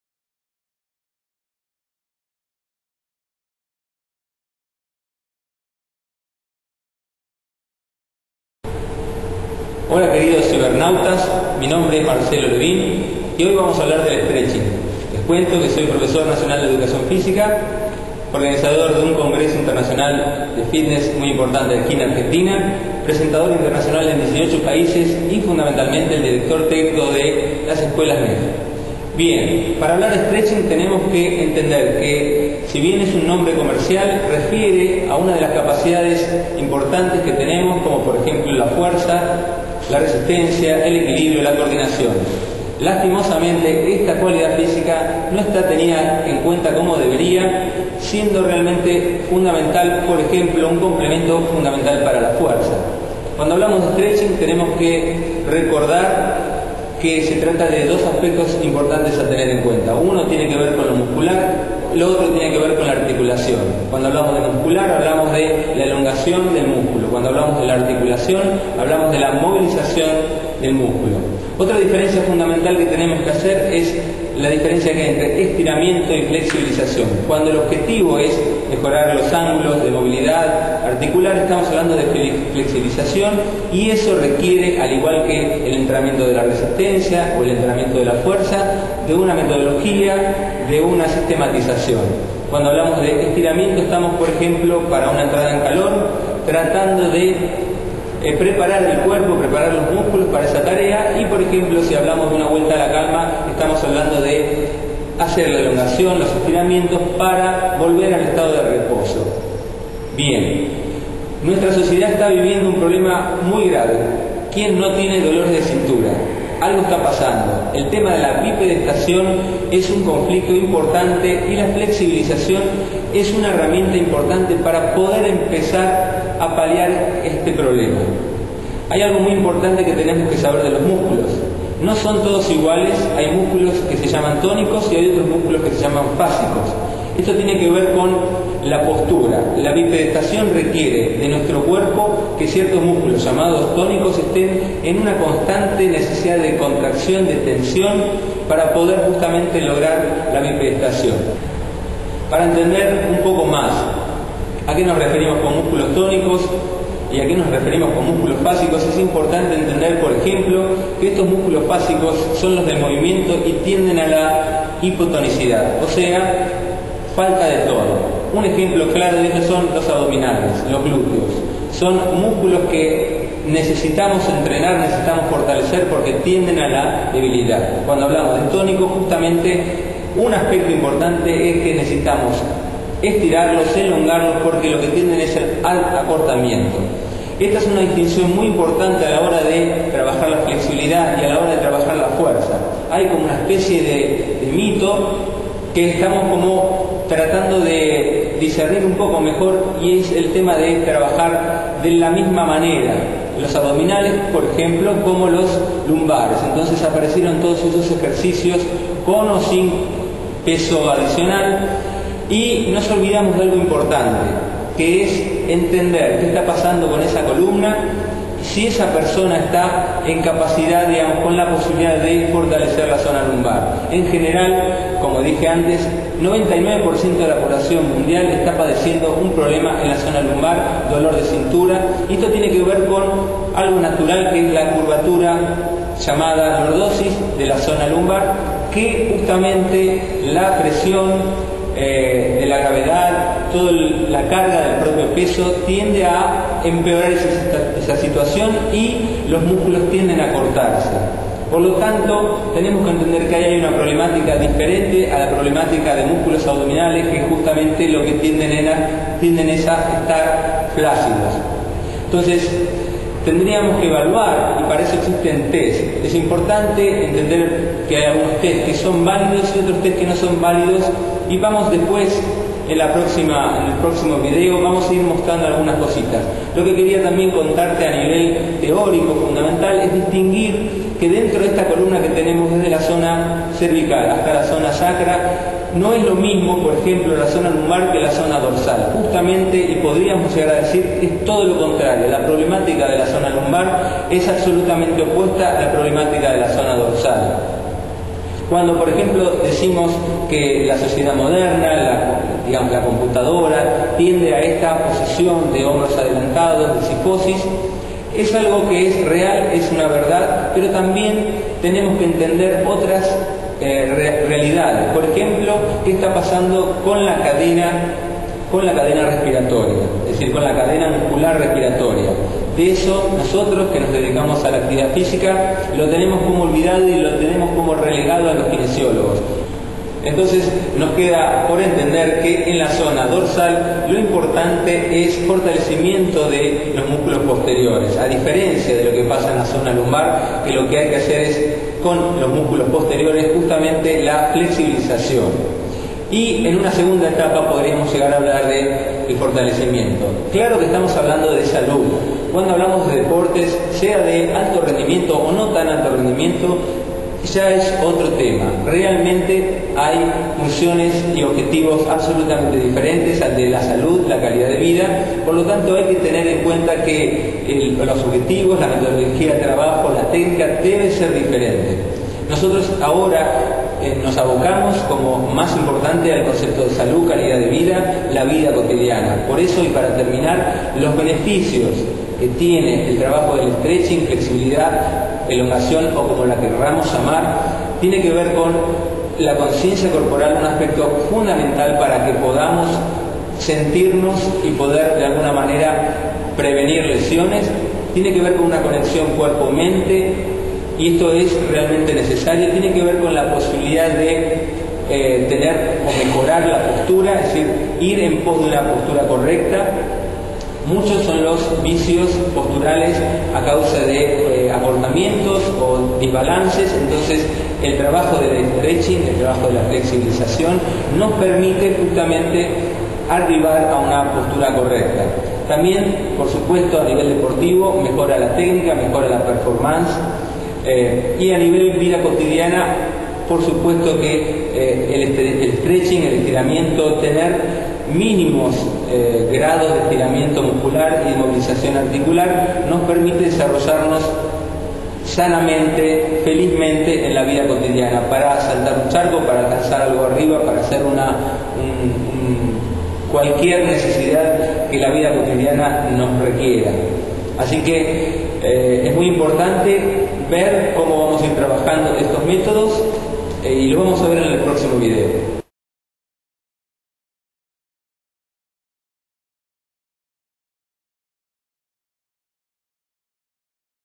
Introducción al fascinante mundo del stretching
Marcelo Levin nos brinda una introducción al fascinante y controvertido mundo del stretching